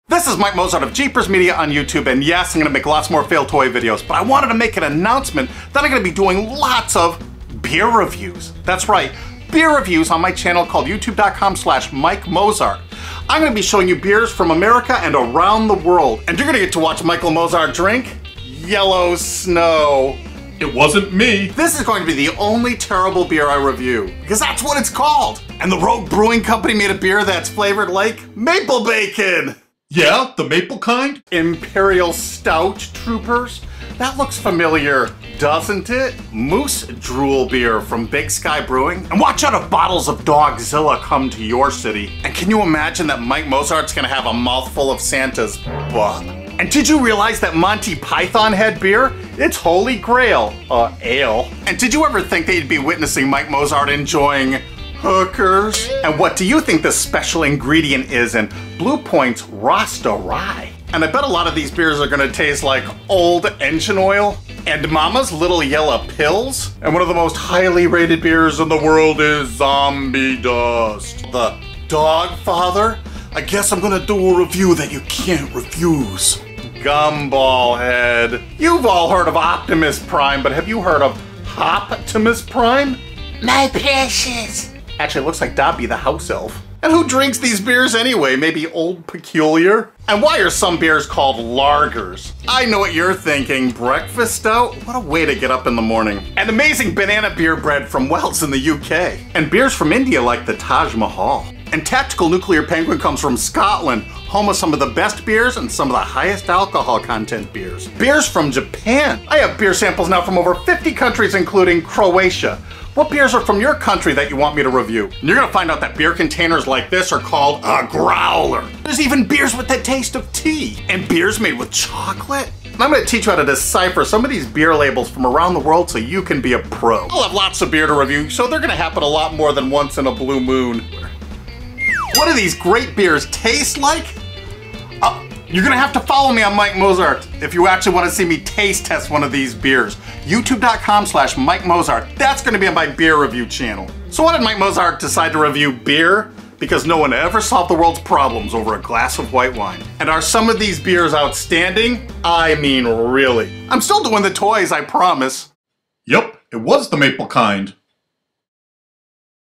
NEW! Beer Reviews with a Funny Twist? By Mike Mozart of JeepersMedia Beers, Ales Porters and Stout
YES! Beer Reviews from Mike Mozart will begin on his channel "MikeMozart" I'll be viewing Beers Ales, Porters and Stouts from all over the USA and from around the World. This Video features "Yellow Snow" by the Rogue Brewing Company, "Maple Bacon Beer" in honor of "Voodoo Donut Shop of Seattle" also from Rogue Brewing Company. "Hooker Beer" from Connecticut's Famous Beers and Brews from Japan, Scotland, Croatia, USA and more!